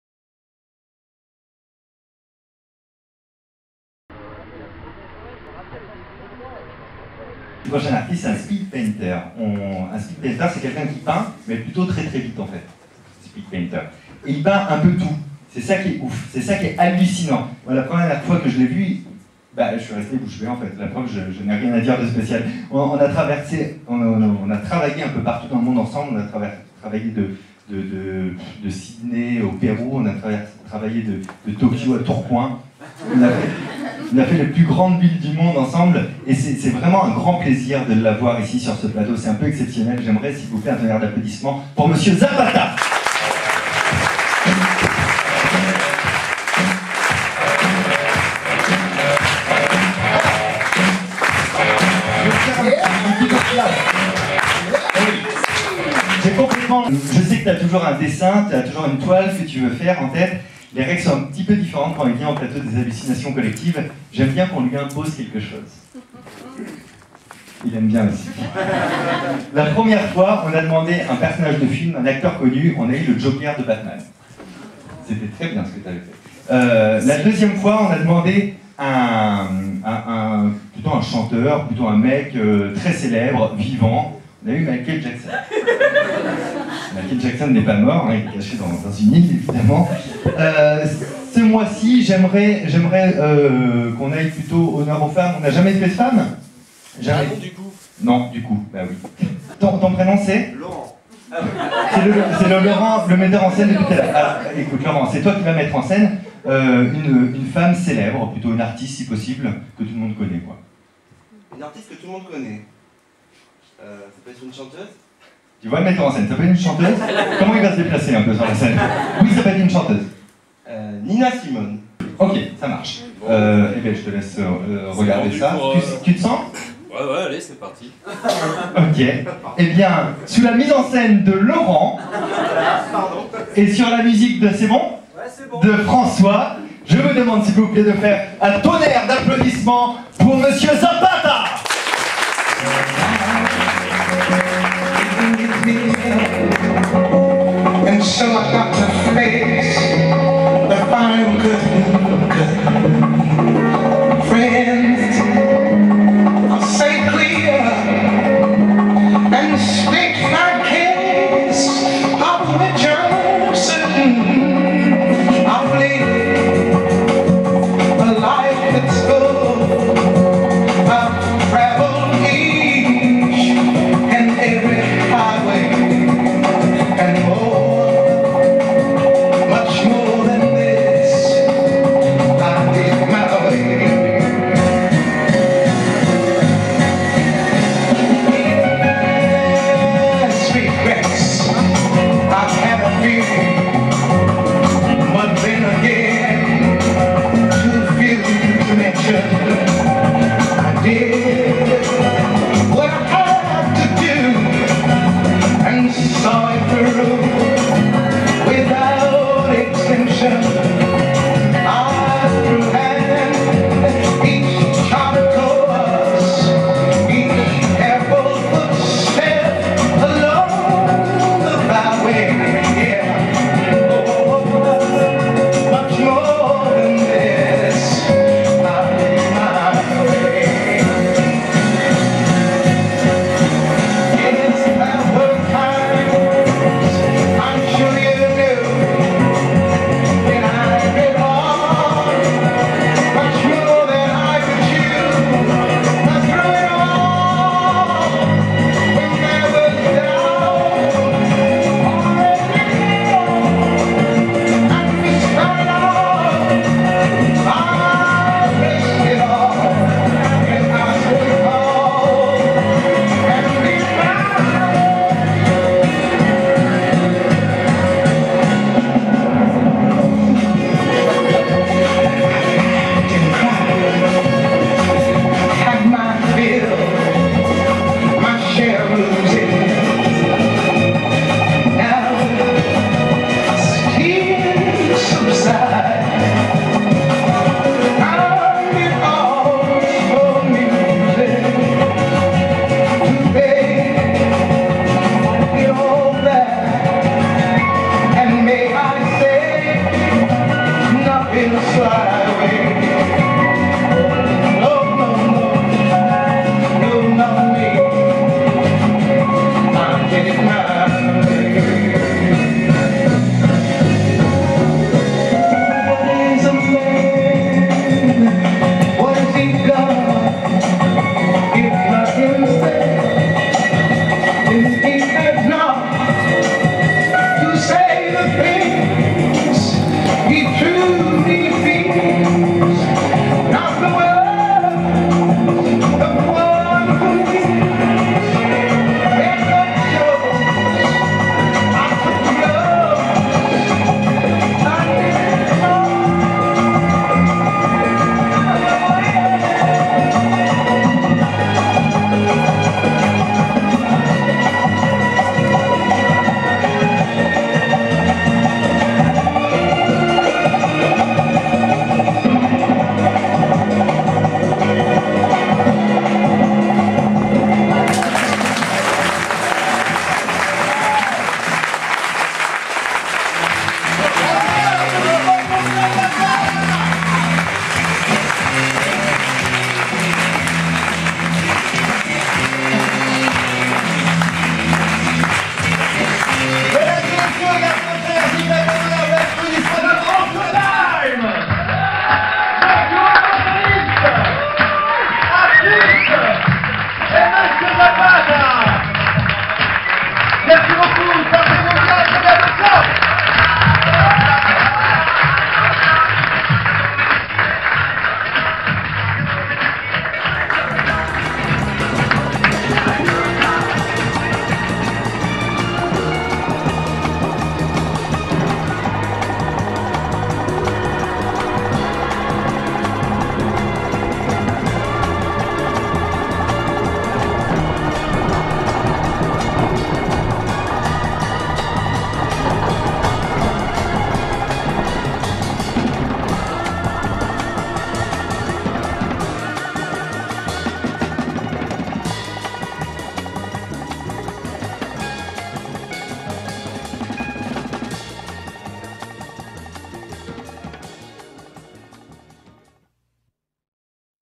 Nina Simone par le peintre performer Zapata
Le portrait de Nina Simone en 4 minutes par le peintre performer Zapata au Grand Point Virgule lors de la 7ème édition des Hallucinations Collectives et Autres Illusions Fantastiques, de Fabien Olicard